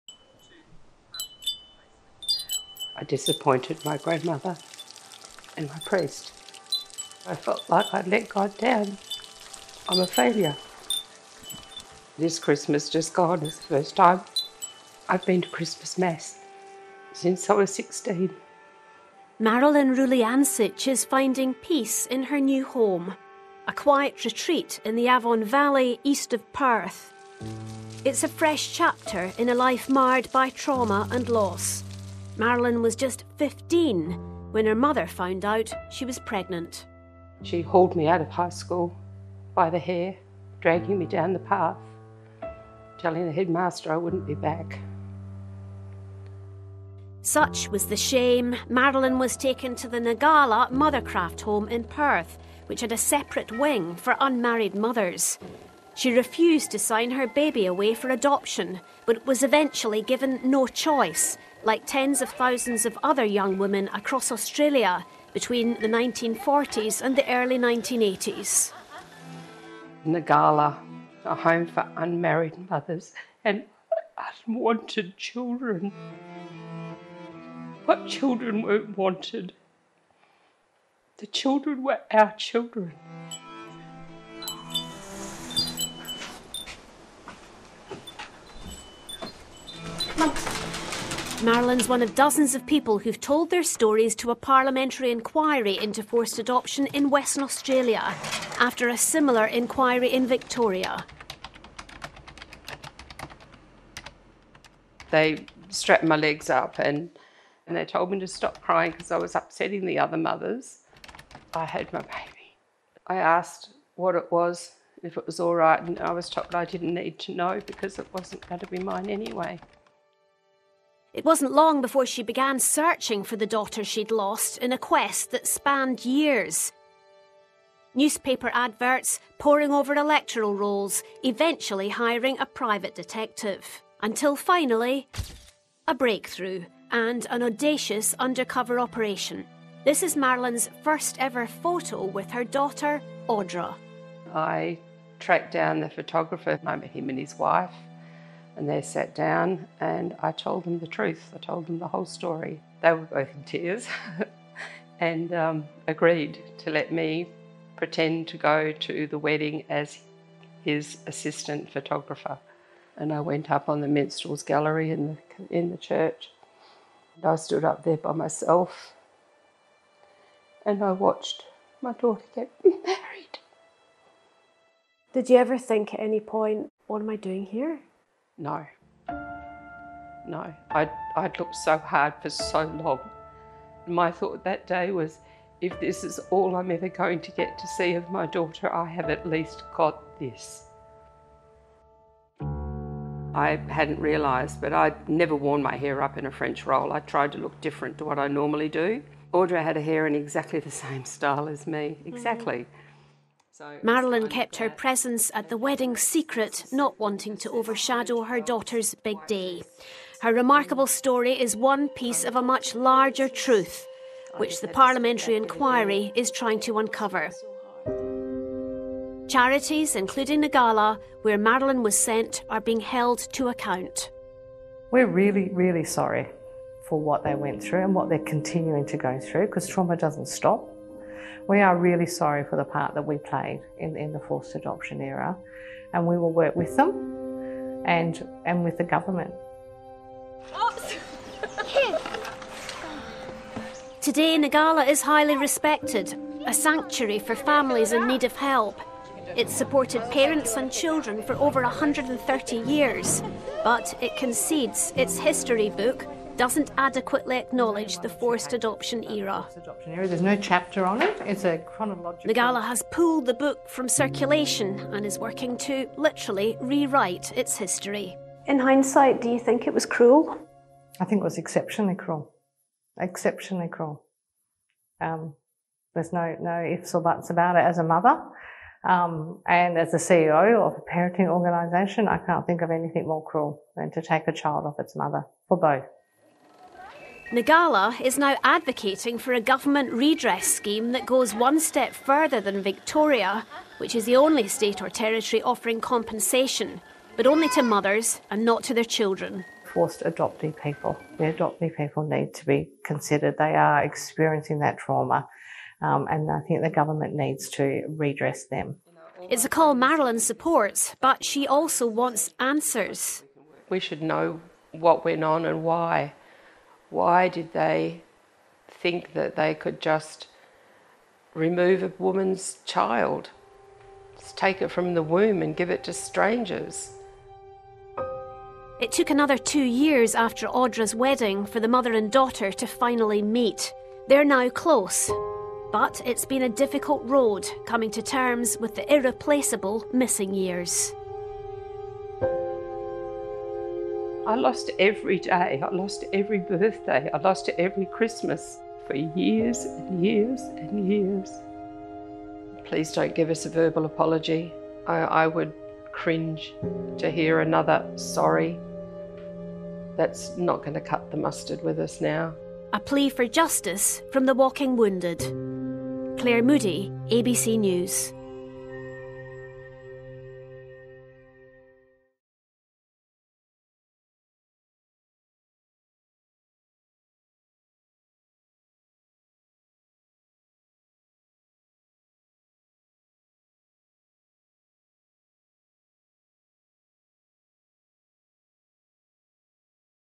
Marilyn's baby was ripped from her at birth, but she never gave up the search for her. | ABC News
Marilyn Rulyancich's baby was taken from her at birth, but she never gave up hope of finding her — hatching an audacious undercover plot to realise her dream.